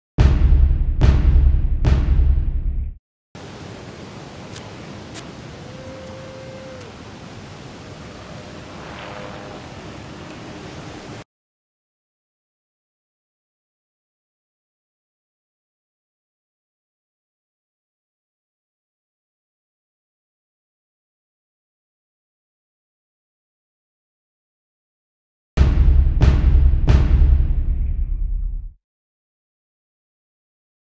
Paige Alms at Jaws 1 - 2017 Billabong Ride of the Year Entry - WSL Big Wave Awards
Paige Alms (Haiku, Hawaii, USA) briefly takes flight on a magic carpet ride and sees it through en route to winning the women's division of the Pe'ahi Challenge at Jaws, Maui, Hawaii on November 11, 2016.  Video by WSL Video Team . An entry in the Billabong Ride of the Year category of the 2017 WSL Big Wave Awards.